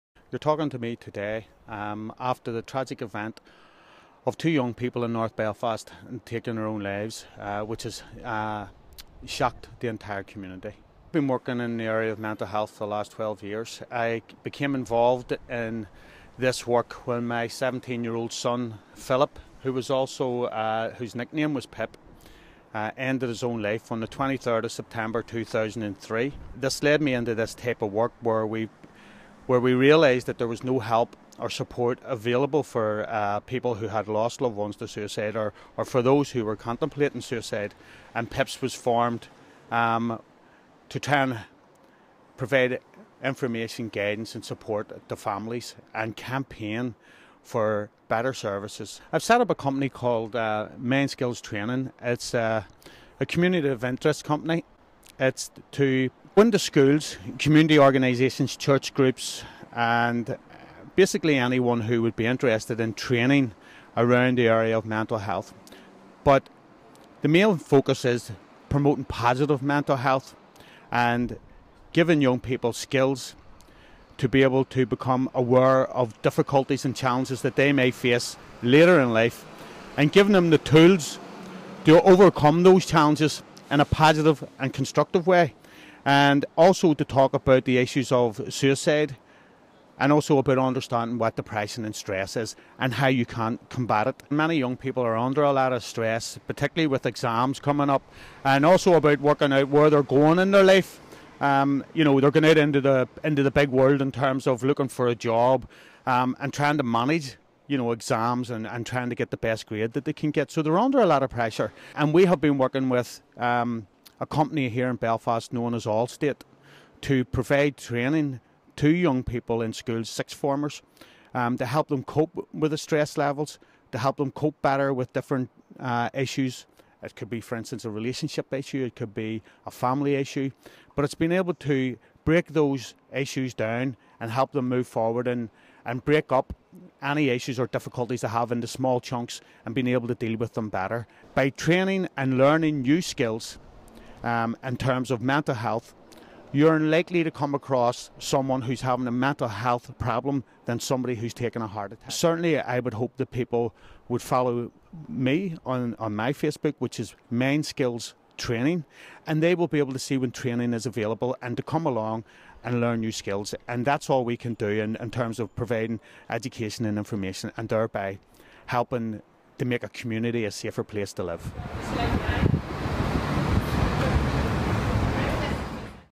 Interview with Philip McTaggart PIPS Mindskills Training
Philip McTaggart of the suicide awareness charity PIPS talks about their work in the wake of yet more young suicides in Belfast.

Interview by video journalist Kirsten Elder. Filmed on an iPhone 5